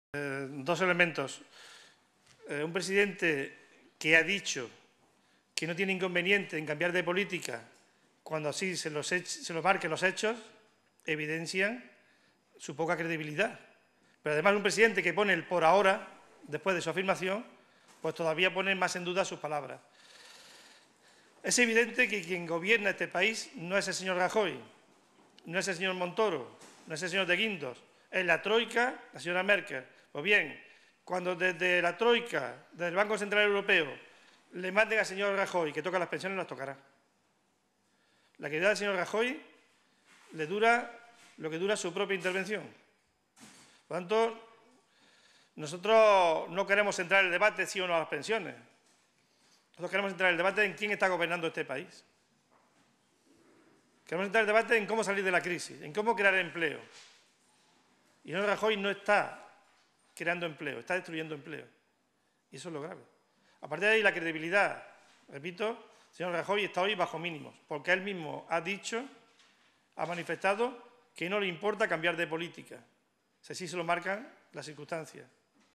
IU dice que es la troika la que gobierna España
El portavoz adjunto de la Izquierda Plural (IU-ICV-CHA) en el Congreso, José Luis Centella, ha arremetido contra el Gobierno y ha apuntado que quien está dirigiendo el país no es "Rajoy, ni Montoro, ni De Guindos, sino la troika y Merkel".